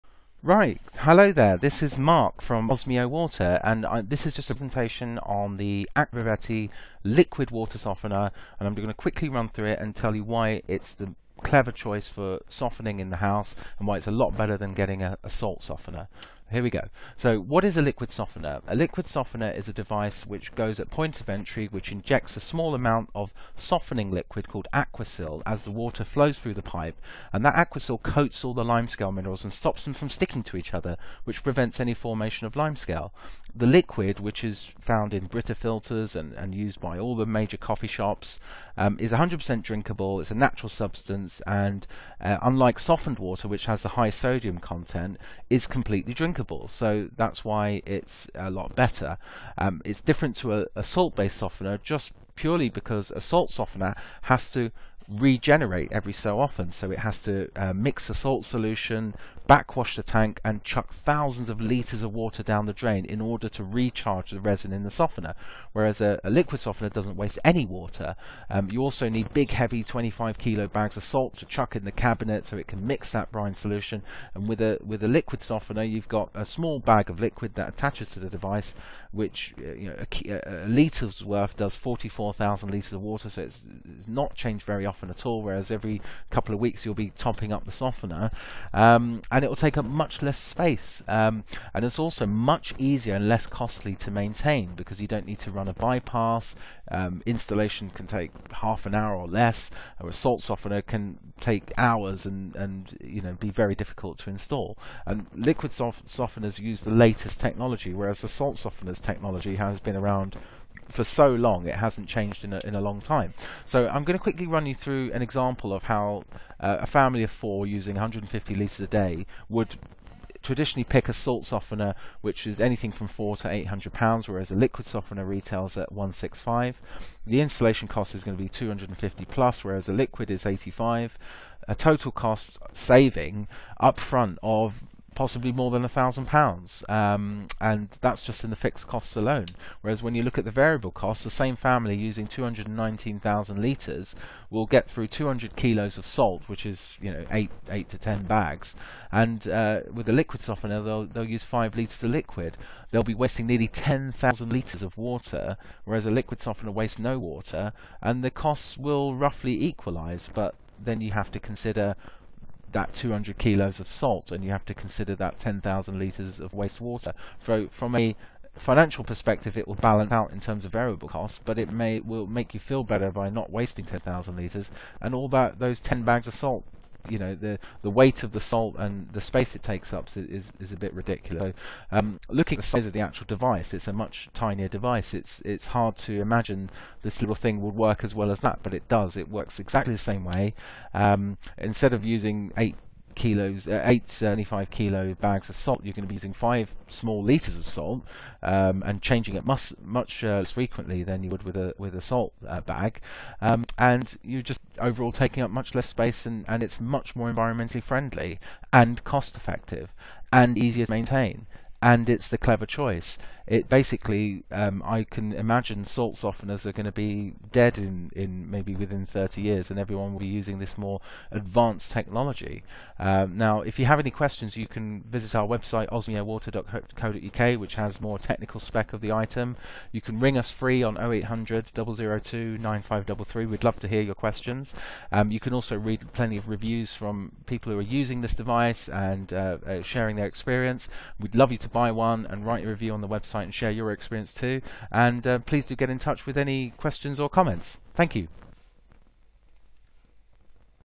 Why a Liquid Water Softener is better than a Salt Water Softener
We explain why a liquid water softener is so much more cost effective, easier to maintain and more environmentally friendly than using a salt based water softener.